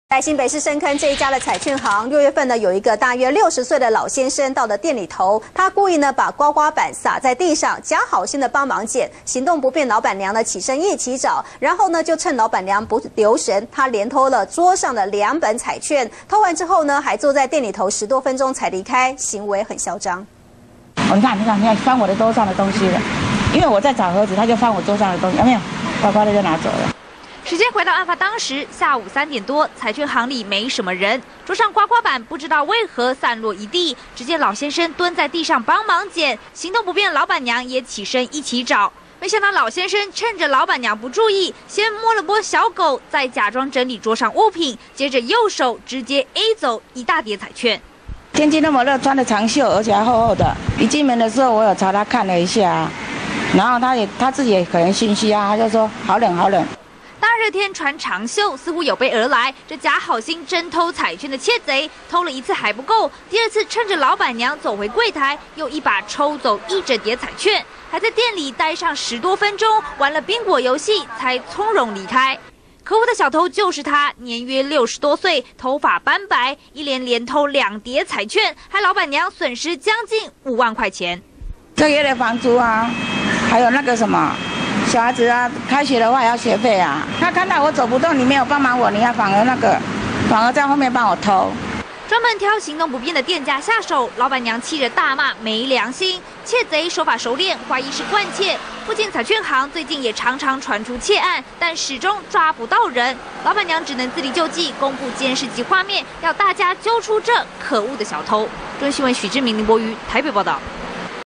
假好心真偷竊！翁幫撿東西 趁機A彩券
中天新聞》新北市深坑一家彩券行，6月份時有一位老先生前往店裡頭，他故意將刮刮樂撒了一地，假好心要幫忙撿起，行動不便的老闆娘一起身，這名老先生就趁她不注意的時候，趁機A走彩券。